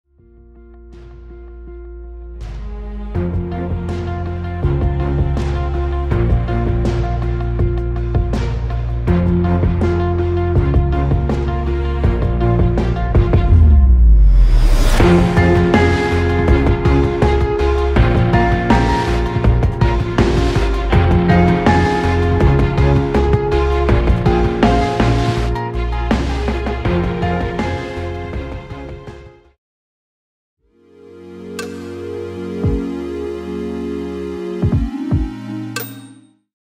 Timelapse 🎥 - Bellagio Fountains ⛲️ - Las Vegas, Nevada 🇺🇸
Timelapse of Bellagio Fountains shit using GoPro10 in Night Timelapse mode. January 2023